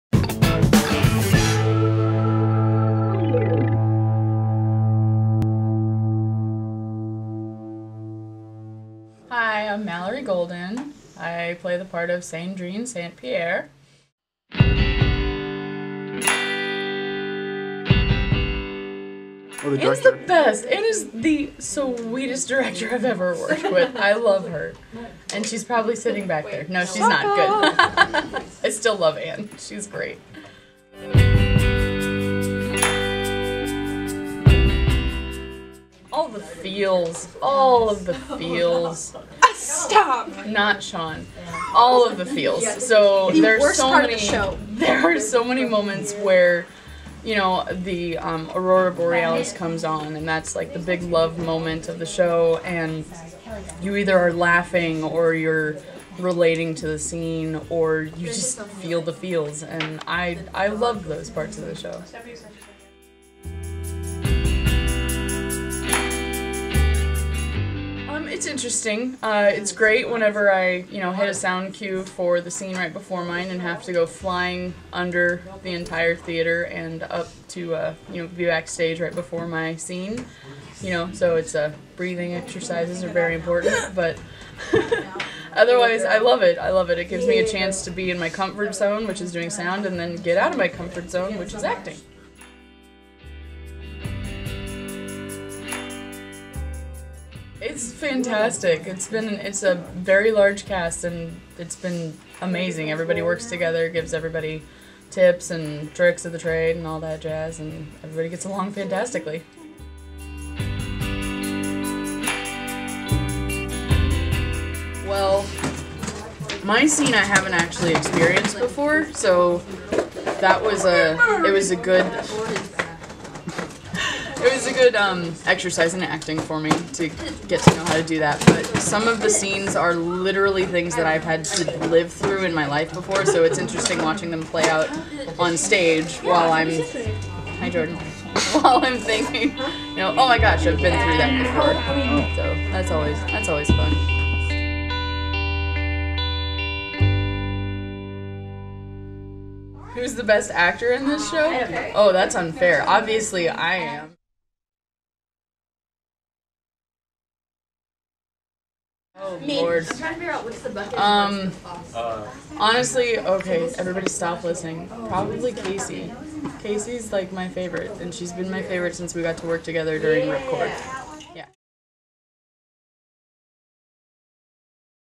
Behind the Curtains with Mallory Golden Almost Maine
Go behind the curtains with Mallory Golden, actress and sound designer for Almost Maine.